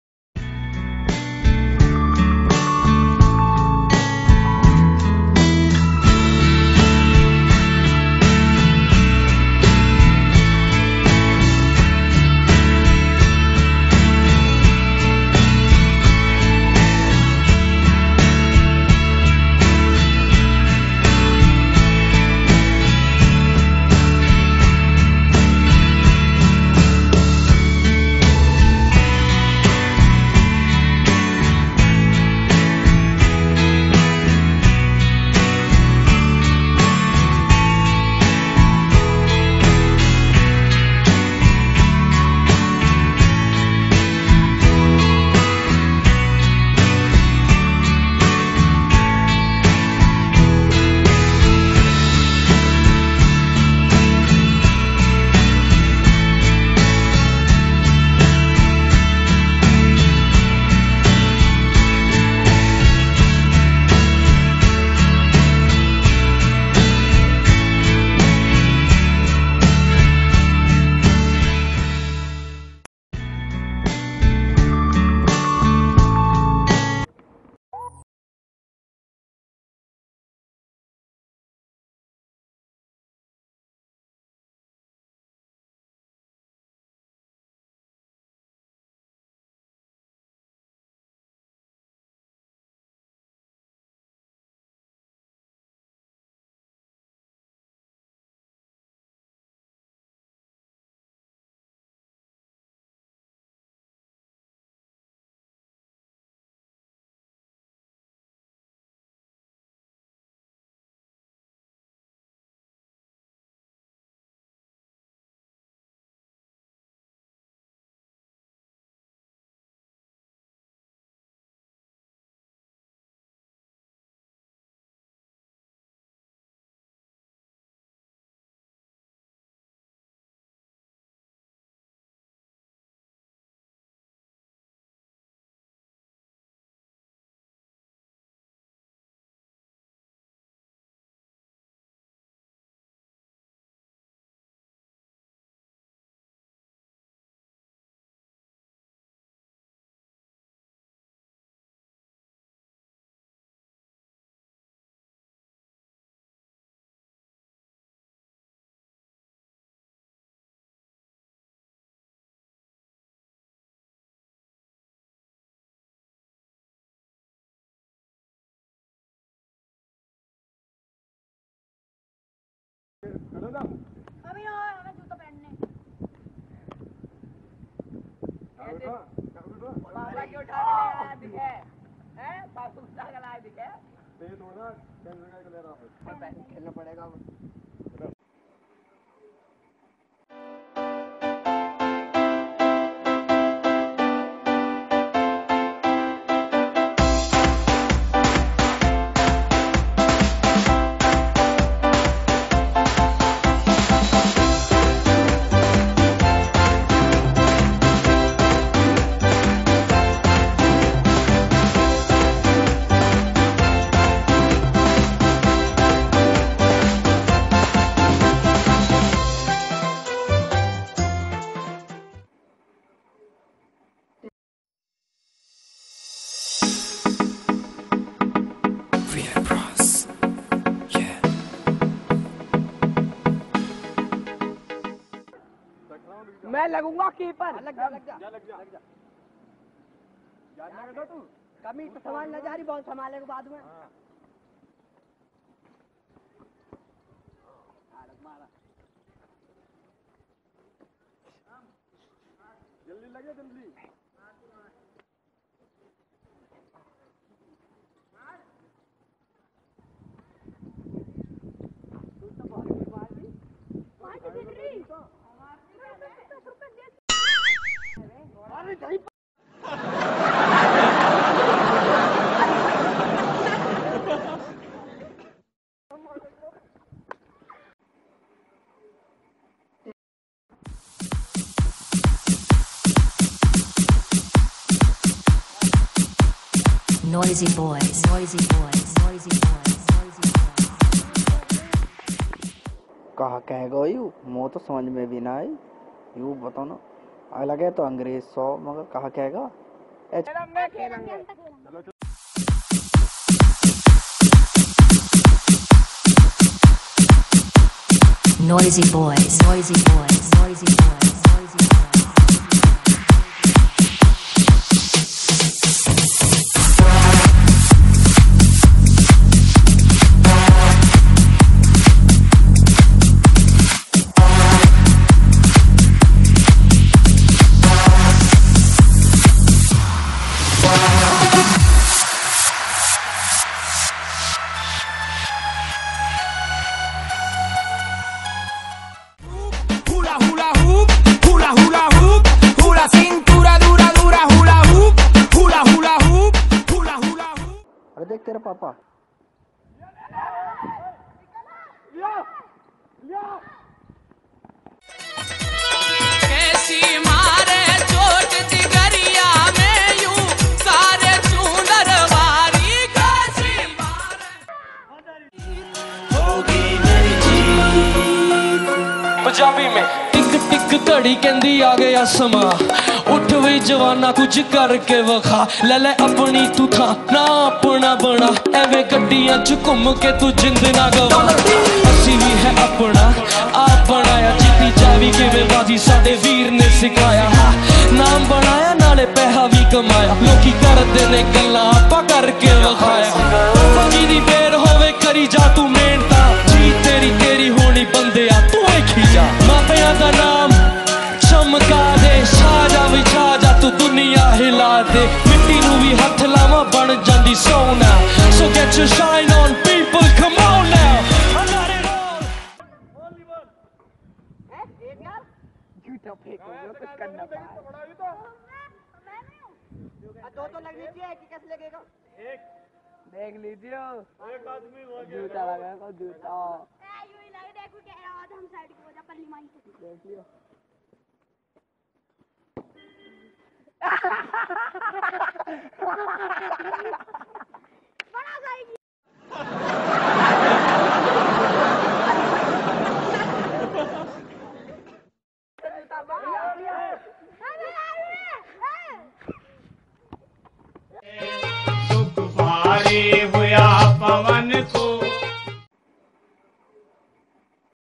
Desi vs professional footballer (funny video)
In this video we are made some footballers like desi and professional we made this video work hard and holding take this video please like us comment us subscribers